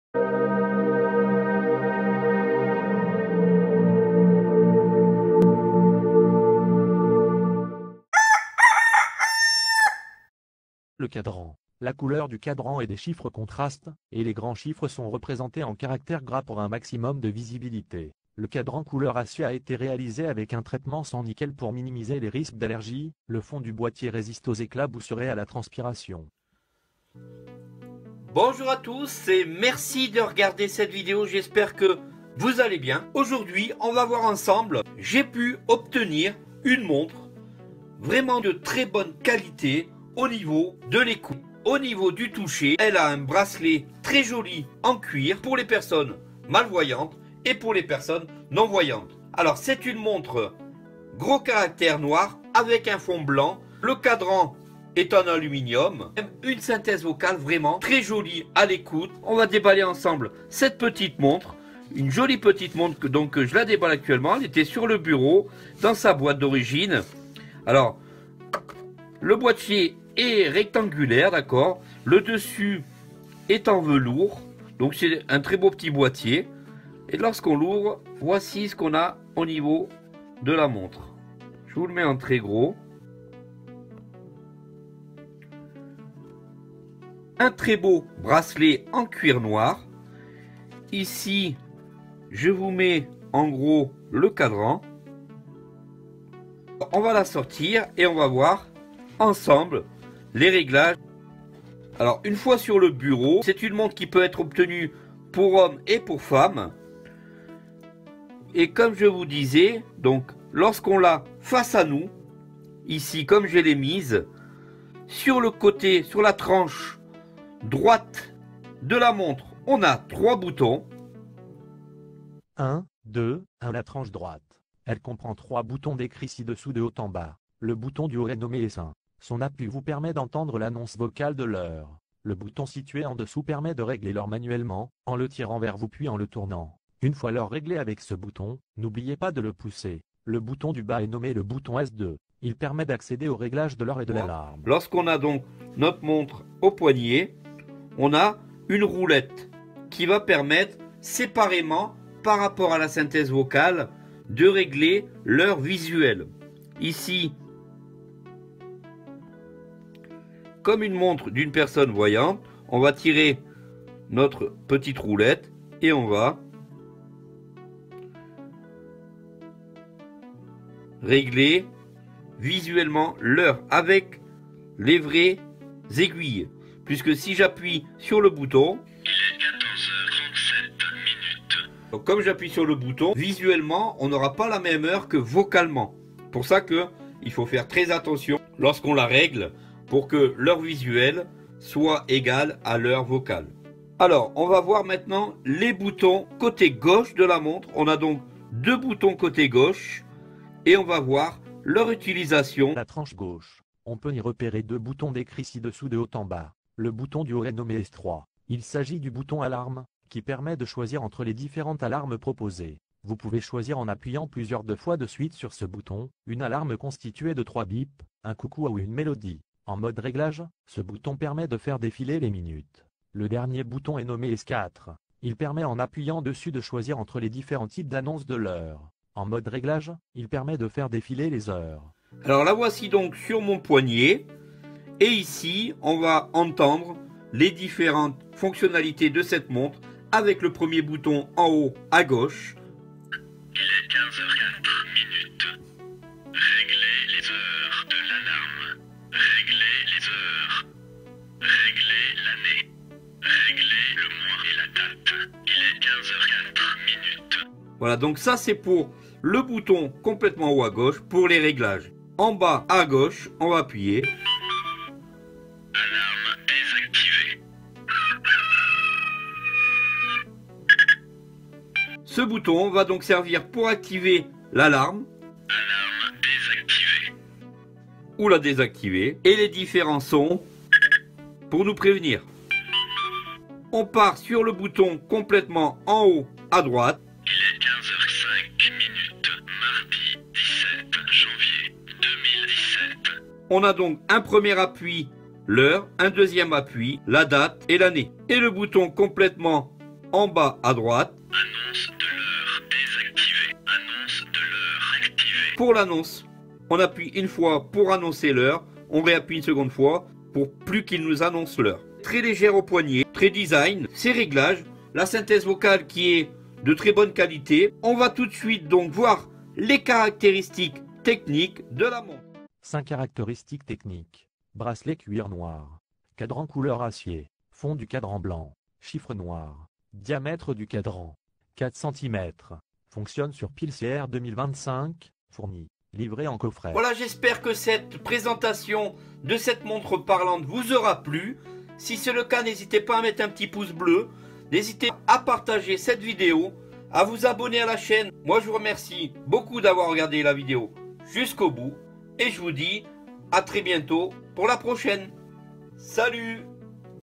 Présentation D'Une Montre Originale Equipé D'une Synthèse Vocale Et Gros Caractère Montre Parlan
Bonjour à tous voici une montre très sympa avec une synthèse vocale super audible elle fonctionne aussi bien en parlant elle possède une molette pour régler l'heure avec les aiguilles normale 4 boutons tactiles pour les différentes fonctions vocale vraiment un beau design merci de d'avoir regarder cette vidéo et de vous abonner à ma chaîne à bientôt.